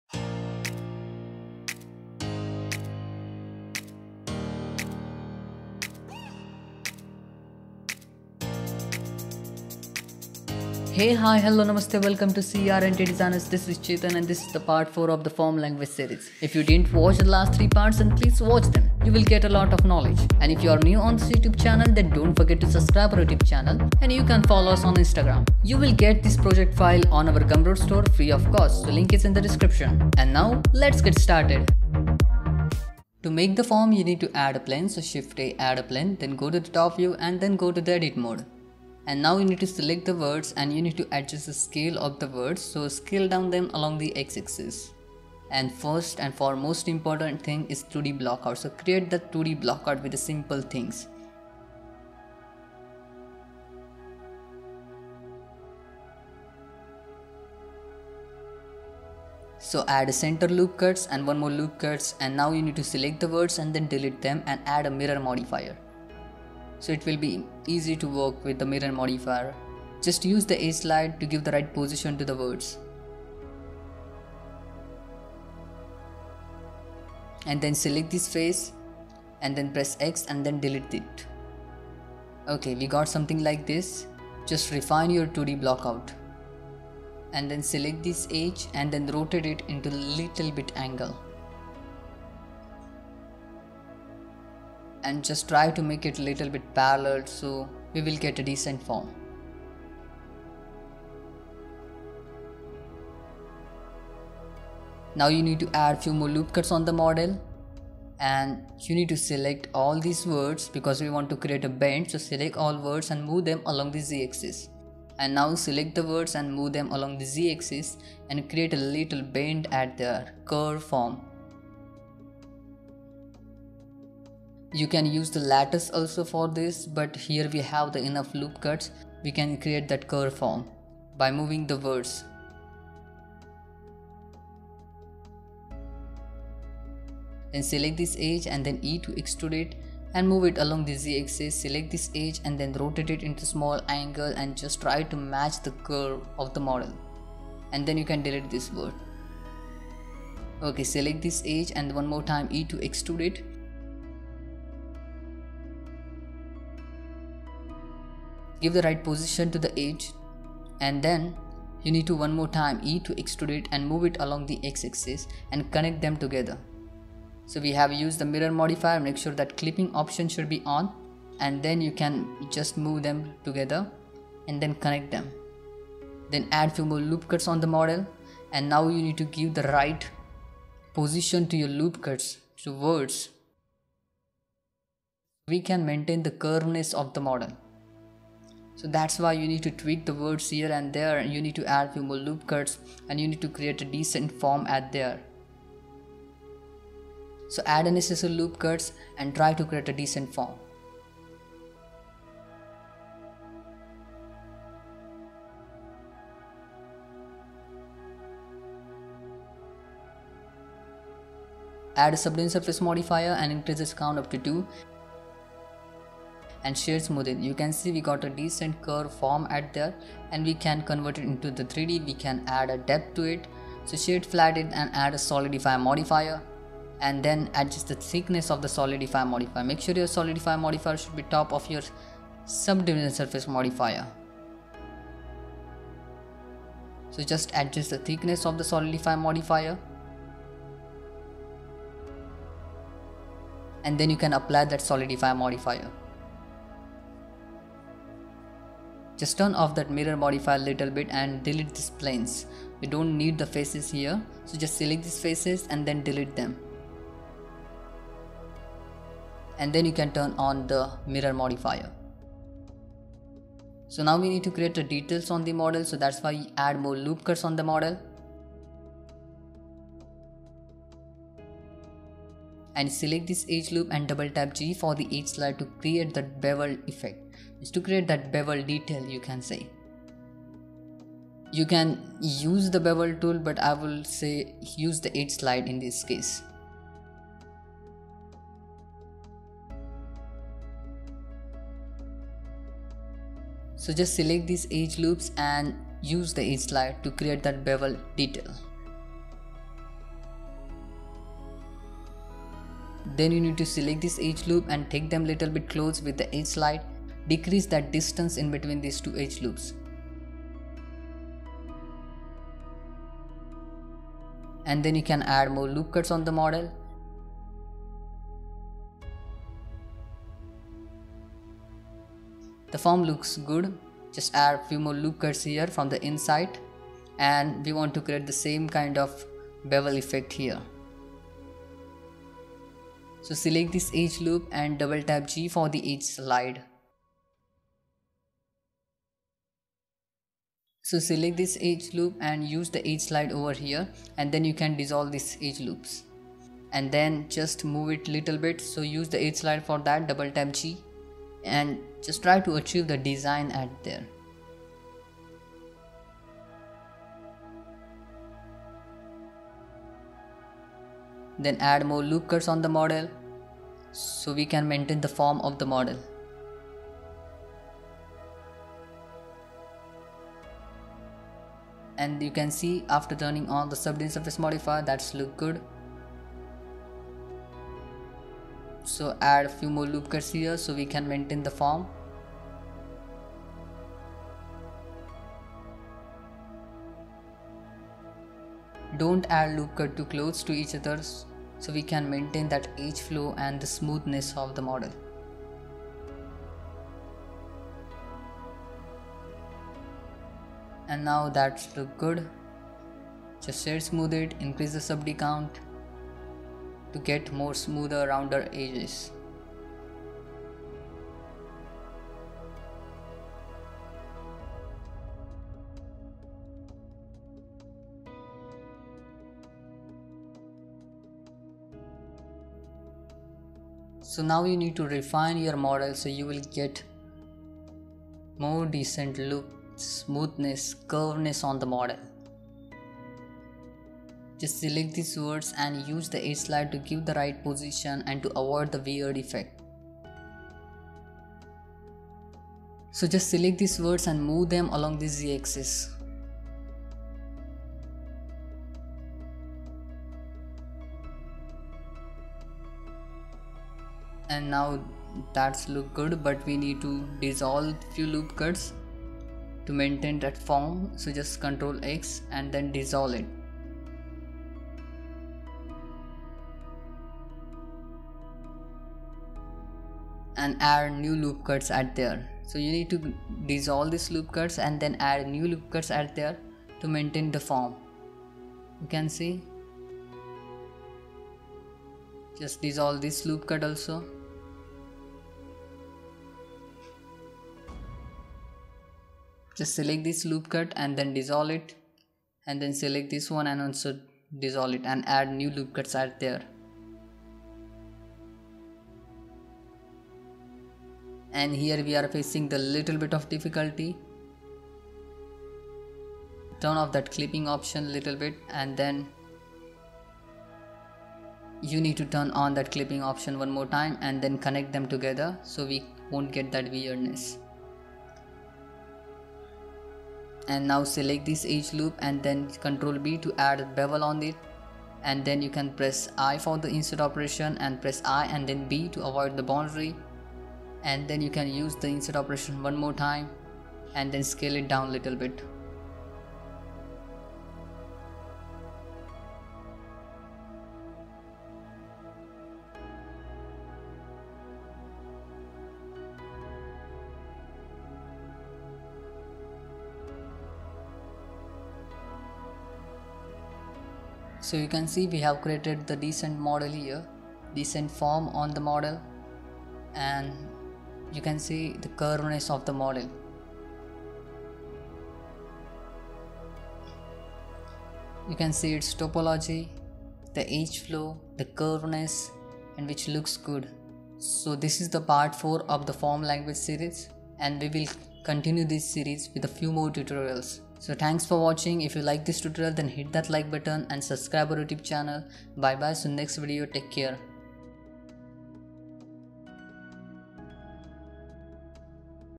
Blender Modeling Guide | Learn Form Language | Part 4 | Blender Tutorial
This blender tutorial series will help you to learn form language. Understanding form language is very important to create 3d art. To learn form language we will create different industrial designs. I hope you will like this tutorial. If you have any questions related to hard surface modeling and blender then you can ask me, I will try to answer your questions.

ABOUT US